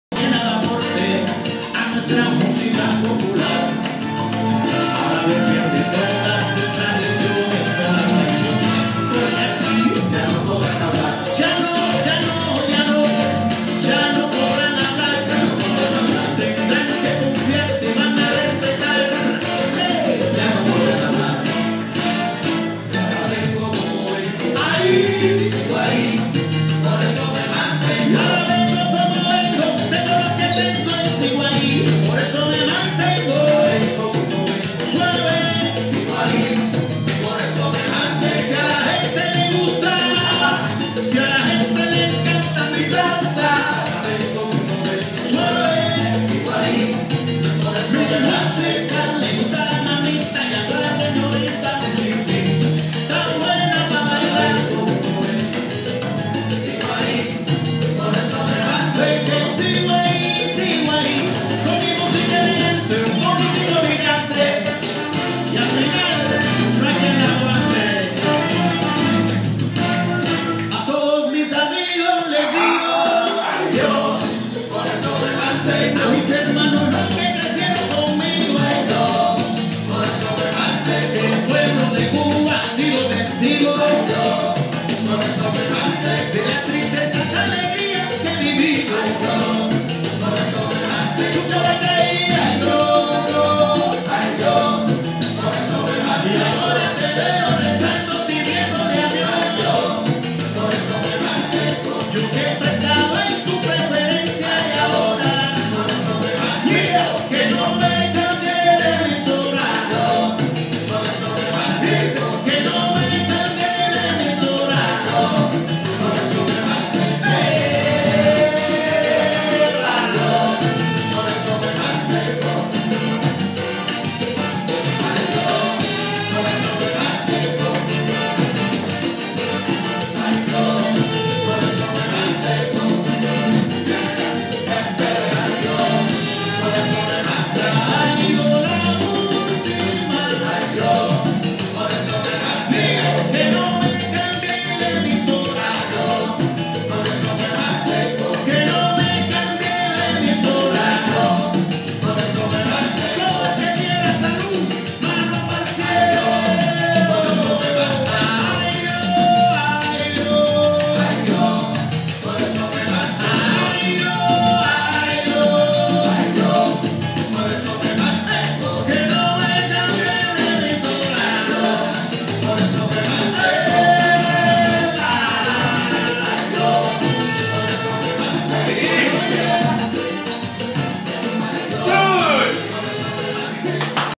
Salsa Class in Cuba with Gilberto bis
Lear Salsa in La Havana with Gilberto, best teacher !!!